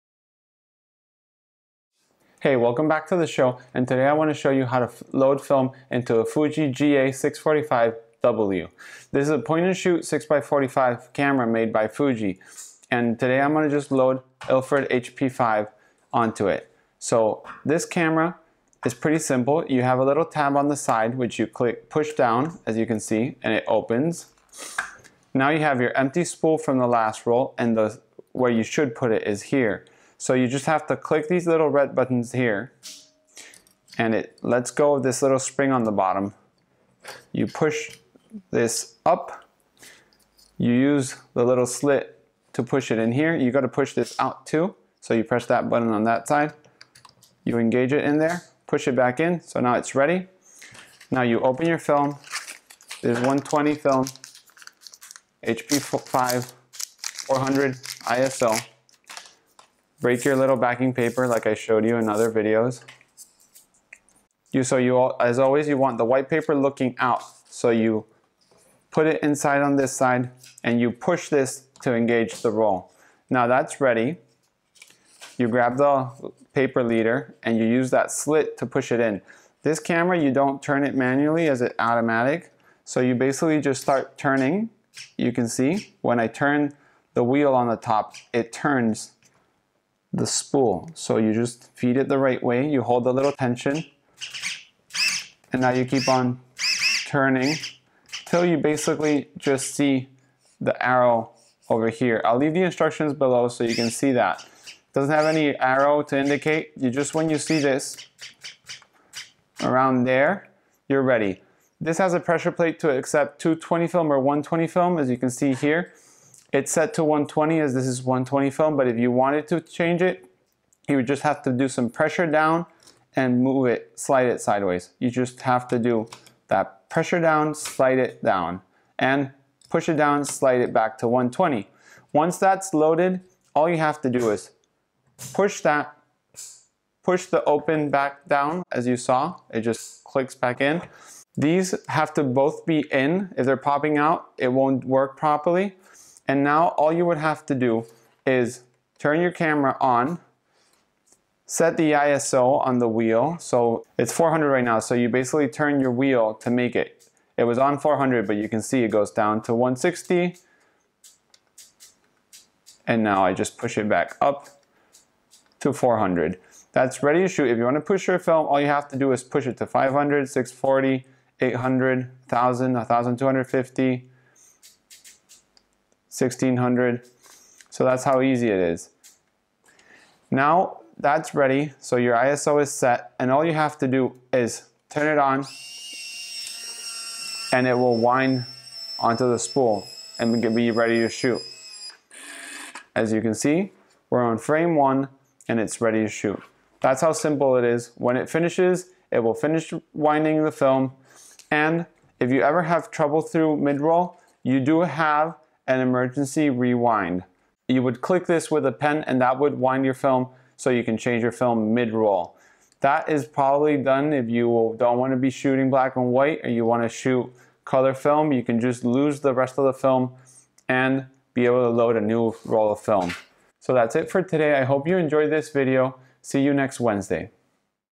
Fuji GA645W || Film Loading
This video shows how to load 120 film in a Fuji GA645W.

Please try to do it in a shaded area. Also if its the first time try using a dummy roll to get the hang of it.